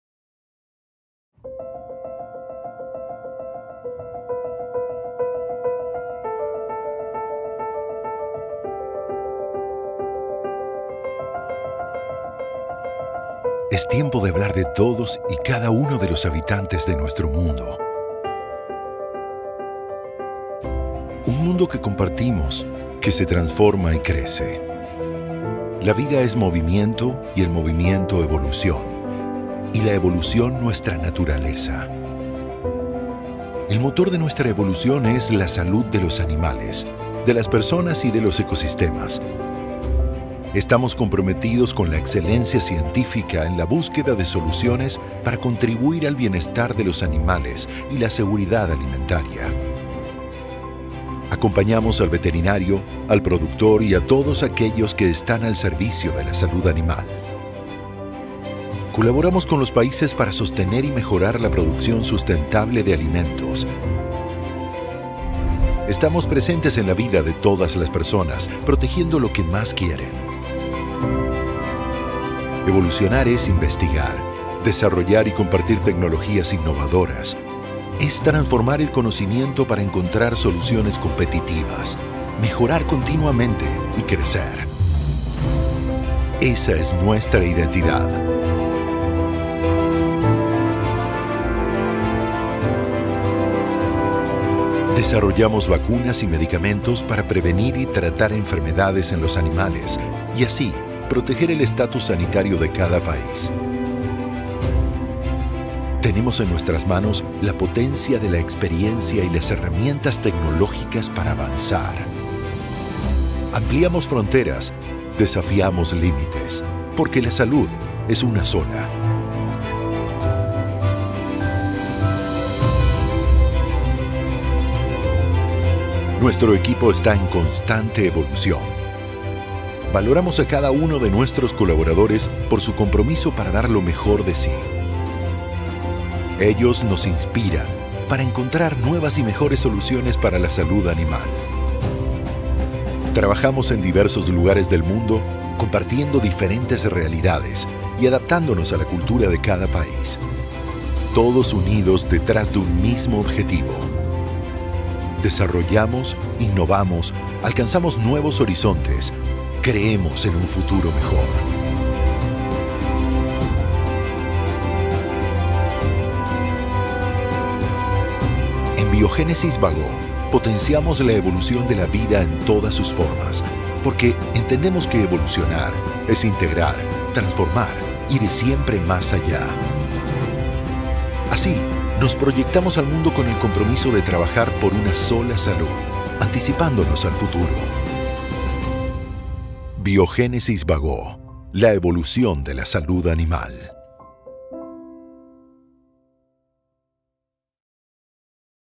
Biogénesis Bagó reafirma su compromiso con la evolución de la salud animal
Basada en el concepto de evolución, la compañía confirma su liderazgo en salud animal, asegurando también el bienestar de las personas y del planeta.

Garín, 16 de marzo del 2023 – Biogénesis Bagó lanza su nueva identidad corporativa reforzando su compromiso con la evolución de la salud animal y el rol fundamental de ésta en el bienestar de las personas y de los ecosistemas.

La compañía de biotecnología especializada en el desarrollo, producción y comercialización de productos destinados a la salud animal dio a conocer su nuevo video institucional en el que reafirma su constante evolución y transformación para responder a los desafíos que plantea la salud animal en el mundo.

“Este video refleja nuestro espíritu emprendedor y la búsqueda constante de nuevas soluciones a las problemáticas de la salud animal en el mundo”, destaca Esteban Turic, CEO de la empresa. “Tenemos una gran historia que nos respalda. Más de 80 años de trayectoria invirtiendo constantemente en investigación y desarrollo que nos ha permitido transferir nuestras tecnologías a cualquier lugar del mundo. Estos logros dentro de la industria fueron posibles gracias al gran equipo que hemos formado y que ha adquirido un profundo conocimiento en salud animal. También a nuestros accionistas que, incansablemente, nos inspiran a ir más allá”.

La compañía implementa una estrategia de expansión global orientada a la diversificación en territorios y la ampliación de su portafolio de productos para alcanzar nuevas especies que le permita ubicarse entre las primeras 20 líderes en la industria de salud animal en los próximos 5 años.

Con un liderazgo fuerte en Latinoamérica, Biogénesis Bagó llegó a Asia hace 10 años y durante el último período logró expandir sus soluciones a Medio Oriente, norte de África y a Europa. Su liderazgo en el aporte de soluciones biotecnológicas para prevenir enfermedades que afectan a la productividad animal, así como a la salud de las mascotas, también es resultado del profundo conocimiento de las necesidades de cada mercado. Esa proactividad y espíritu emprendedor lo ha transformado en un socio estratégico para la protección del estatus sanitario de los países, la atención de emergencias y la producción de alimentos seguros.